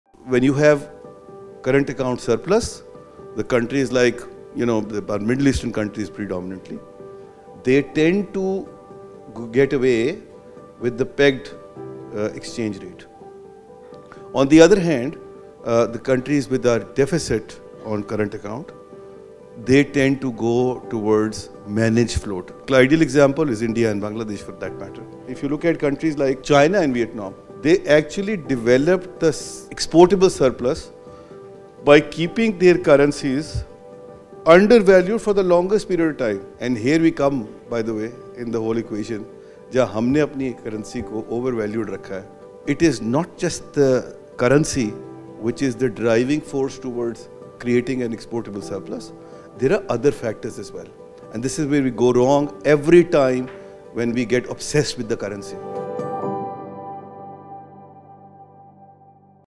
Habib Talks | Mr. Zafar Masud on Managing the Value of a Nation’s Currency | Habib University
Listen to Mr. Zafar Masud’s views on Pakistan's exchange rate policy.